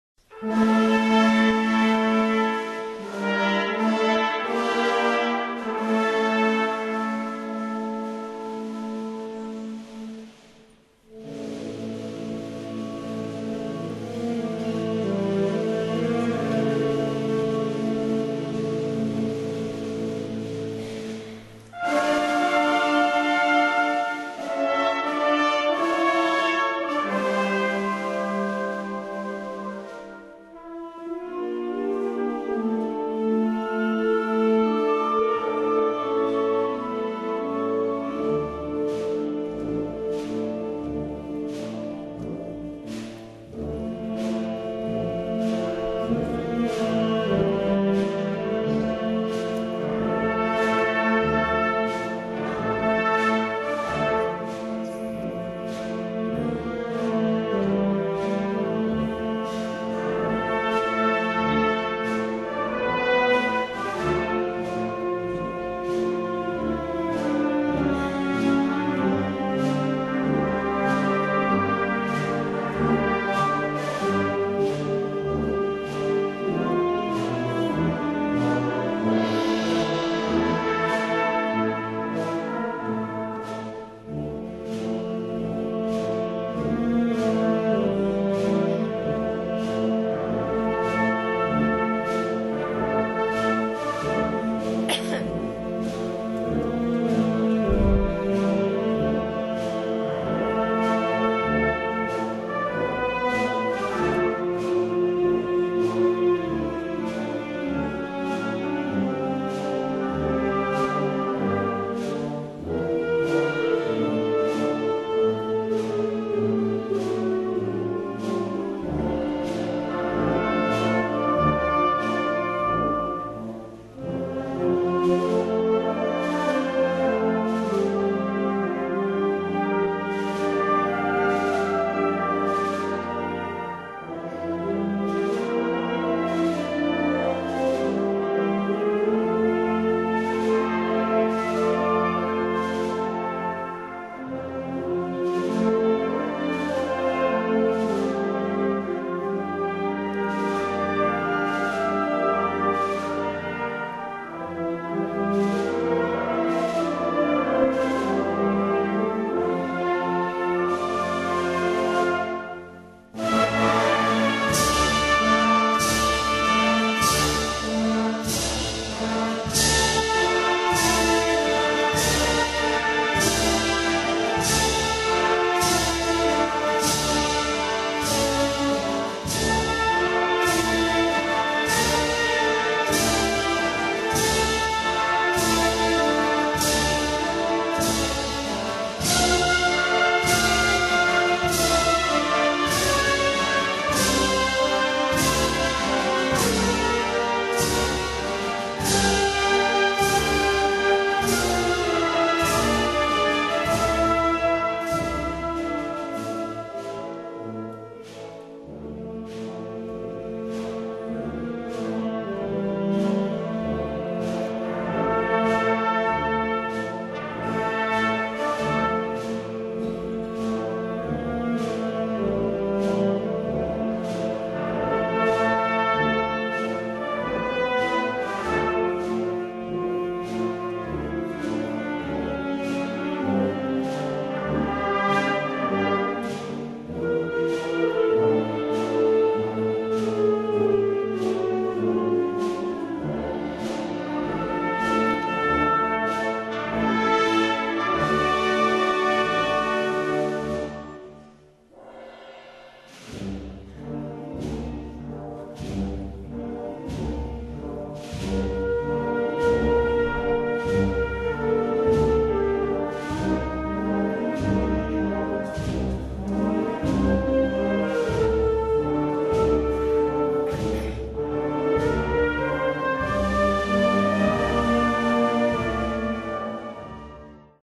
Maria Stma del Consuelo
Salida de la hermandad del Consuelo del Peliron.Semana Santa 2007.